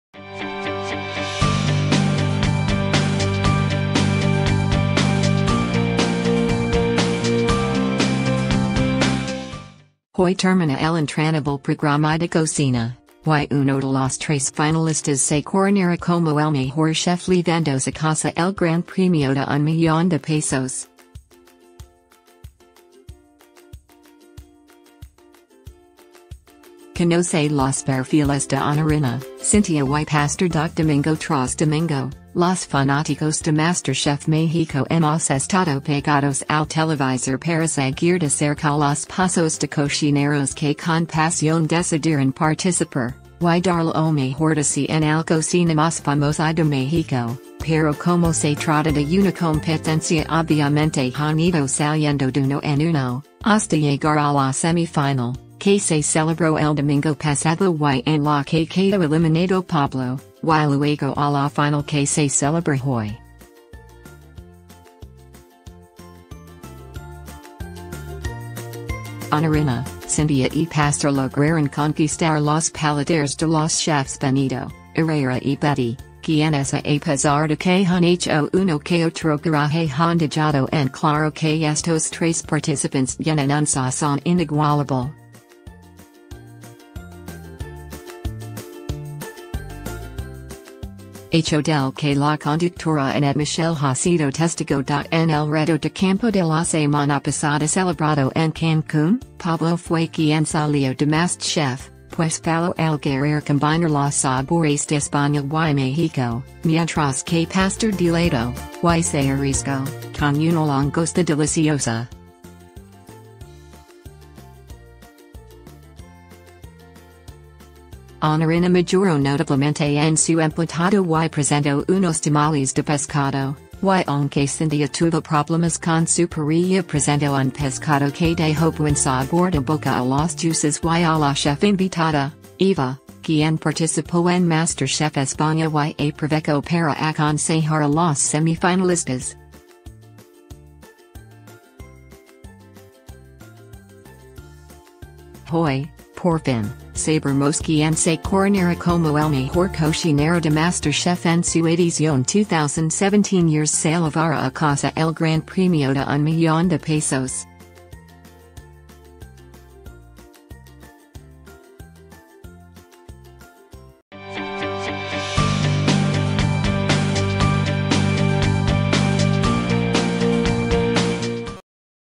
MasterChef... un final con gran sazón
Hoy termina el entrañable programa de cocina, y uno de los tres finalistas se coronará como el mejor chef llevándose a casa el gran premio de un millón de pesos. Conoce los perfiles de Honorina, Cyntia y Pastor.

Domingo tras domingo, los fanáticos de MasterChef México hemos estado pegados al televisor para seguir de cerca los pasos de cocineros que con pasión decidieron participar, y dar lo mejor de sí en al cocina más famosa de México, pero como se trata de una competencia obviamente han ido saliendo de uno en uno, hasta llegar a la semifinal (que se celebró el domingo pasado y en la que quedó eliminado Pablo), y luego a la final que se celebra hoy. Honorina, Cyntia y Pastor lograron conquistar los paladares de los chefs Benito, Herrera y Bety, quienes a apesar de que han hecho uno que otro coraje han dejado en claro que estos tres participantes tienen un sazón inigualable. Hecho del que la conductora Anette Michel ha sido testigo.

Fuente: goo.gl/N9aGsC